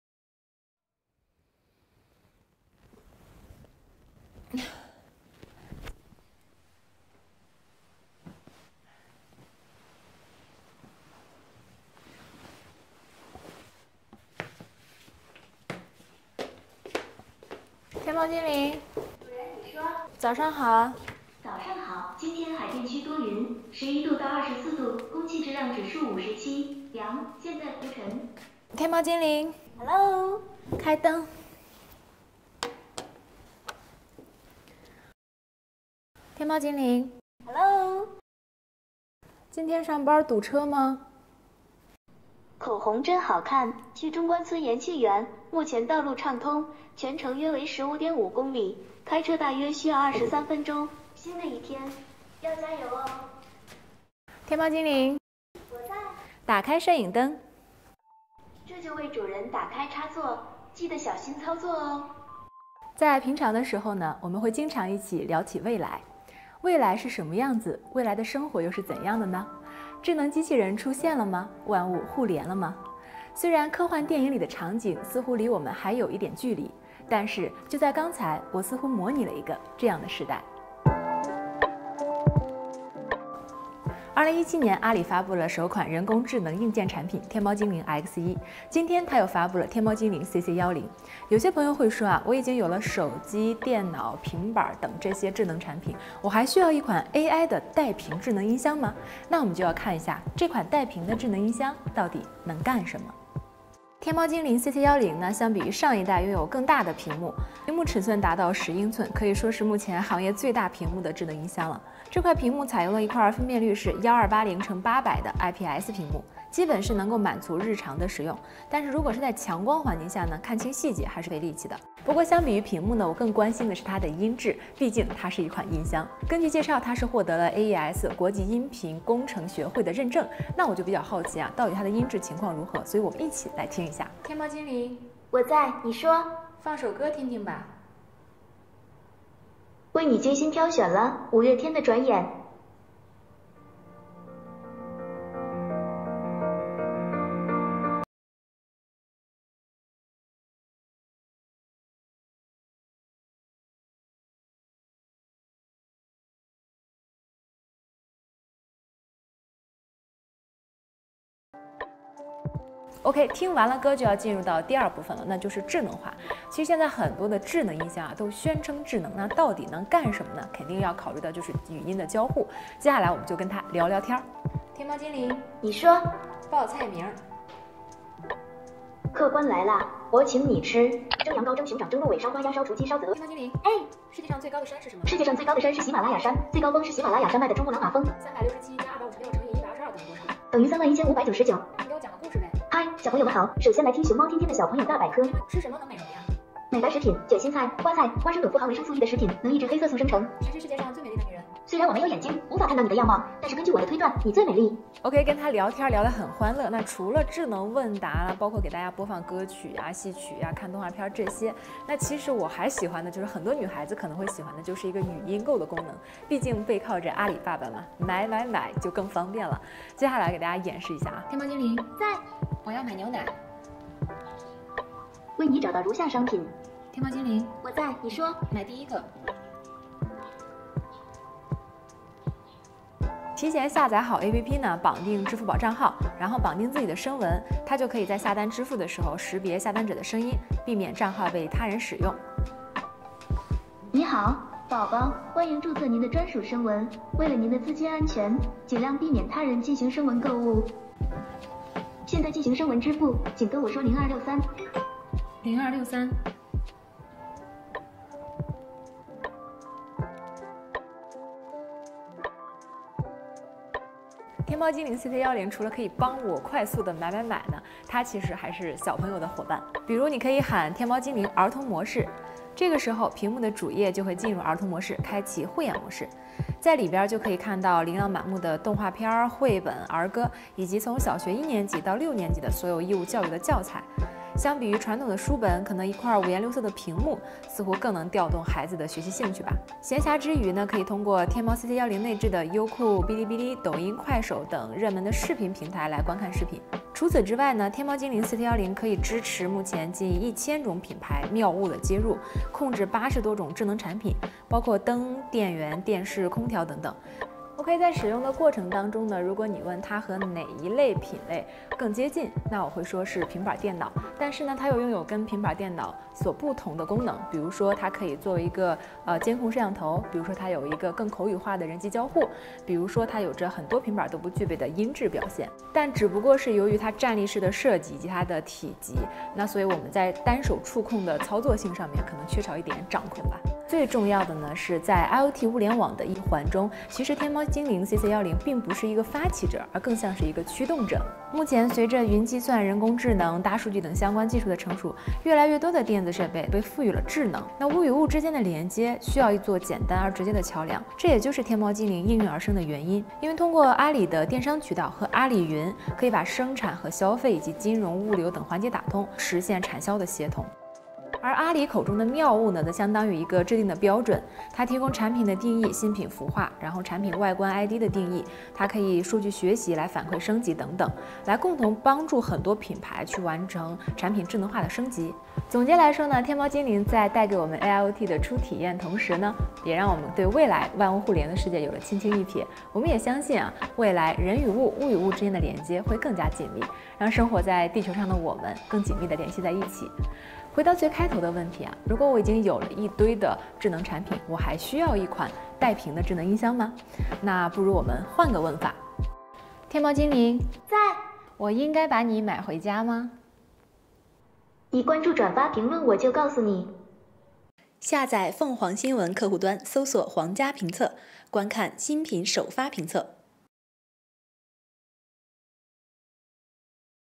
天猫精灵CC10体验：女生最爱 说句话就能买买买 ｜凰家评测
今天下午，阿里发布天猫精灵CC10，这款拥有10英寸屏幕、支持智能人机交互、支持智能家电互联的音箱，能给我们带来怎样的体验？语音购真的方便吗？支持上千种品牌“妙物”后，我们距离现代化智能家居还有多远？本期视频就跟着Eva一起来体验这款天猫精灵CC10吧！

推荐海外用户购买视频里产品的地方，搜然后选择好的信誉的商铺